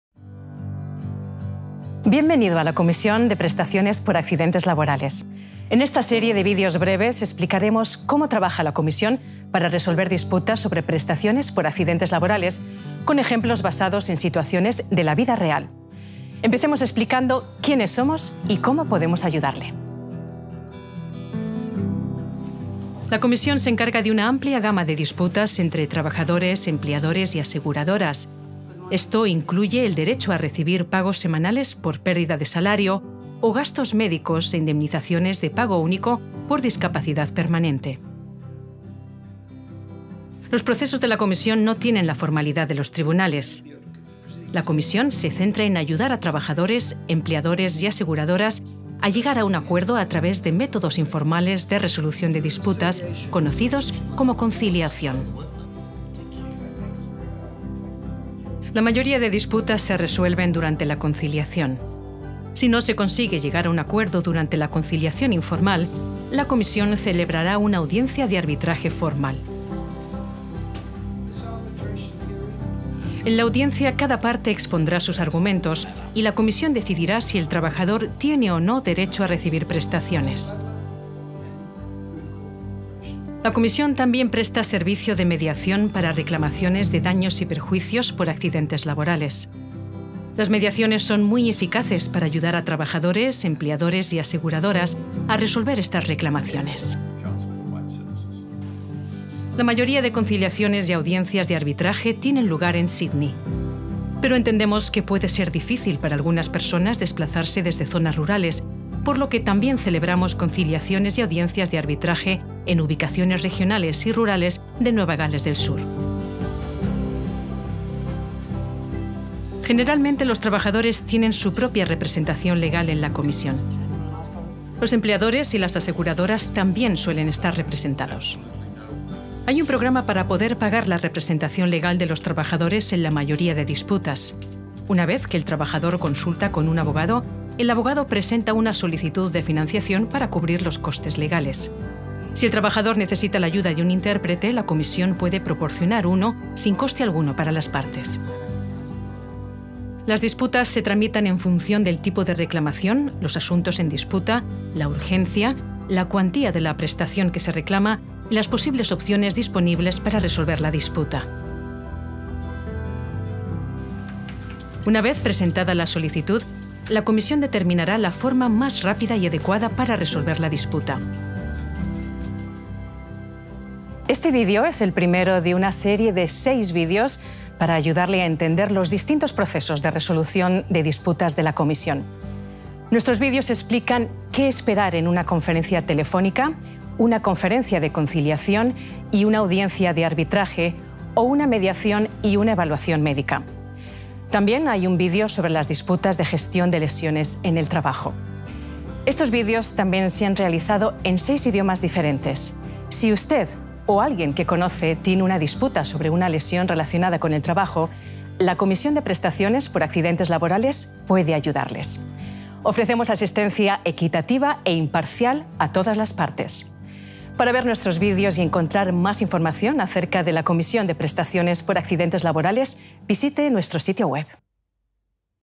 Overview (Spanish) - Workers Compensation Commission
This video is an overview of what to expect when you attend the Workers Compensation Commission. this video is presented in Spanish.